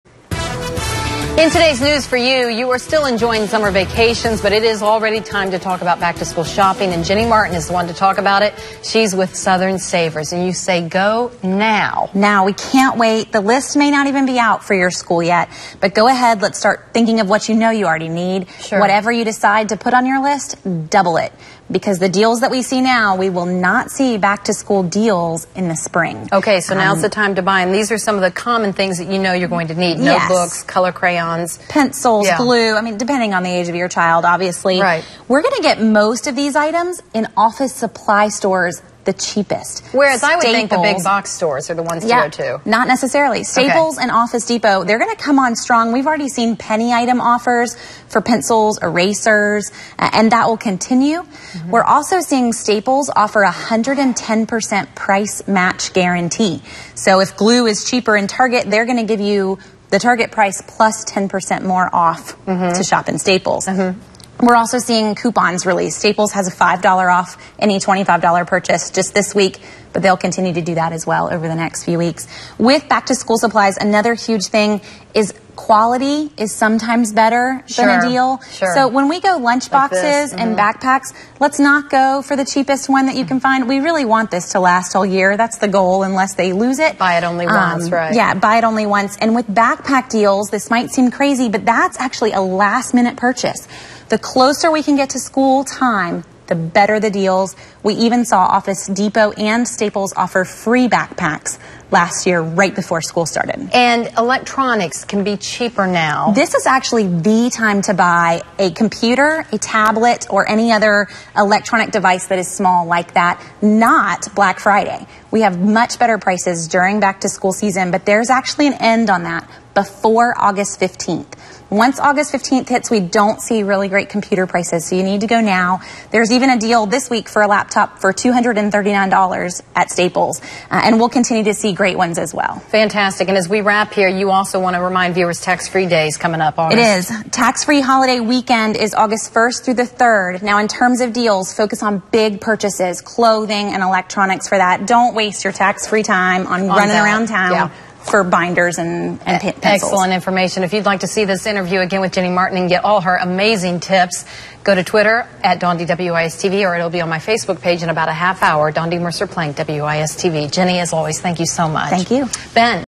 Jenny Martin of Southern Savers on Back to School Savings
Jenny  Martin of Southern Savers tells WIS TV viewers how to buy school supplies for as little as one penny.